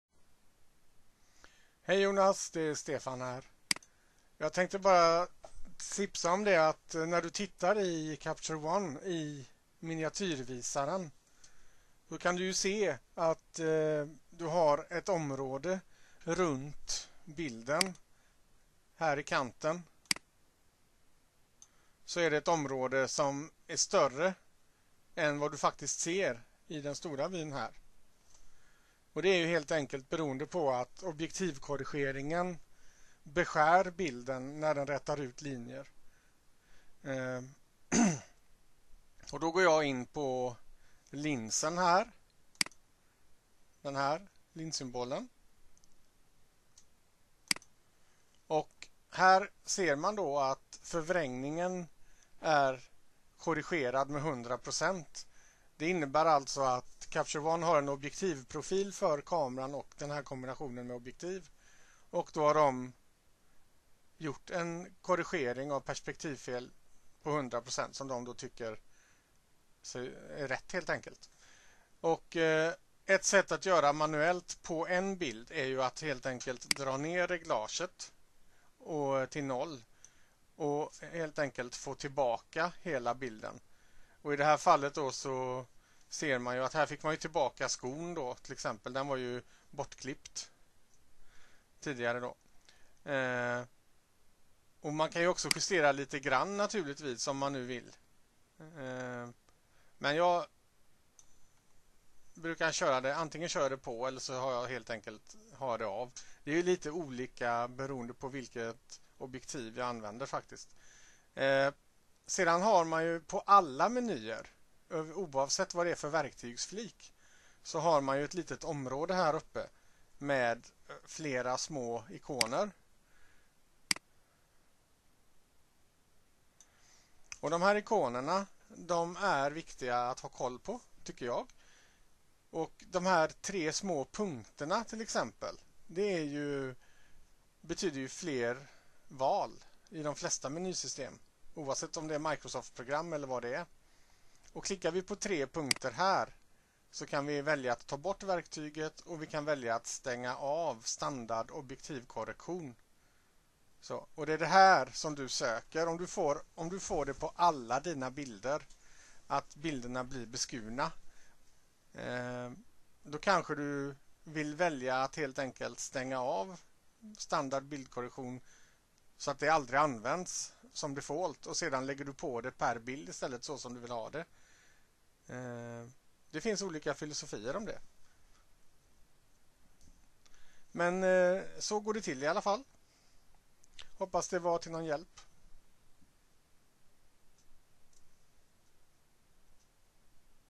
Linskorrigering
Detta besvarar frågan varför vissa bilder blir beskurna i Capture One Pro samt hur man stänger av funktionen linskorrigering som orsakar det.